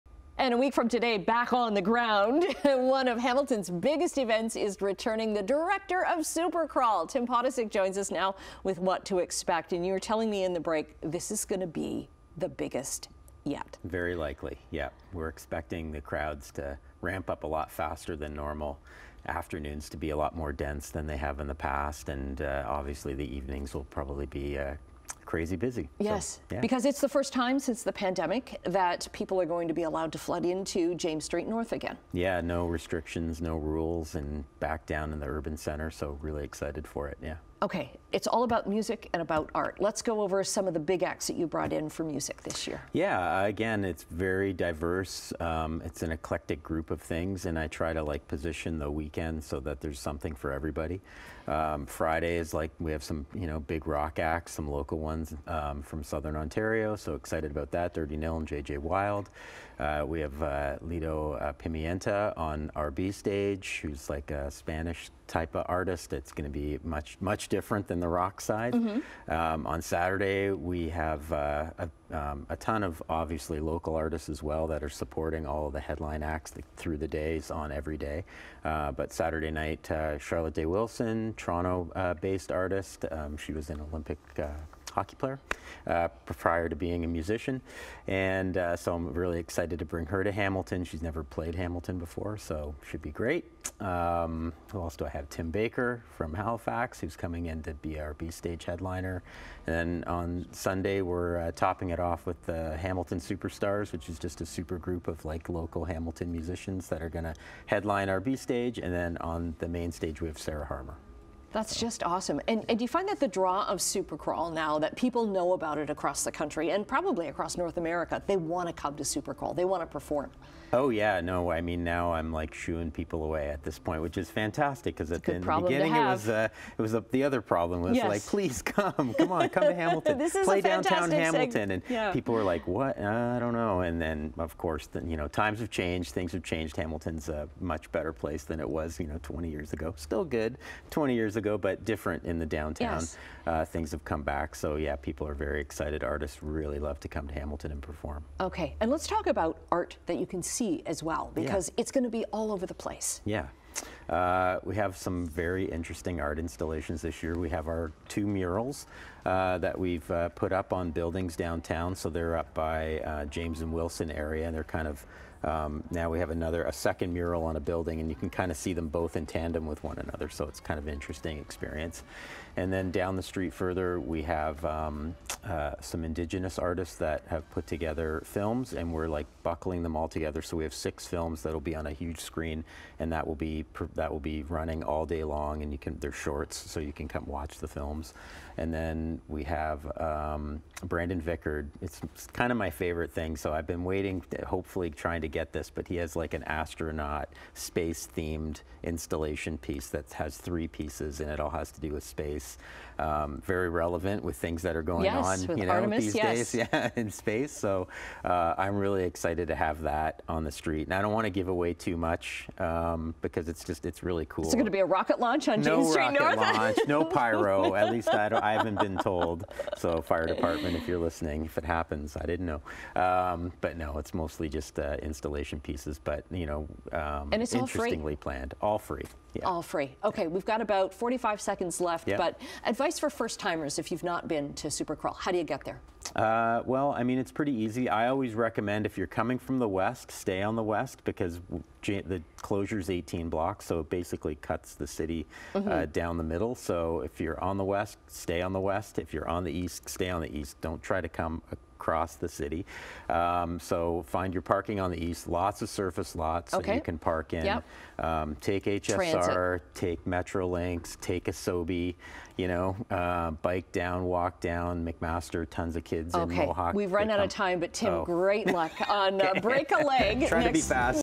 James St. N. will be filled with music & art as Supercrawl is back
This morning we welcomed Supercrawl director, Tim Potocic, who told us more about the event taking place on James St. N. Friday September 9th- Sunday September 11th.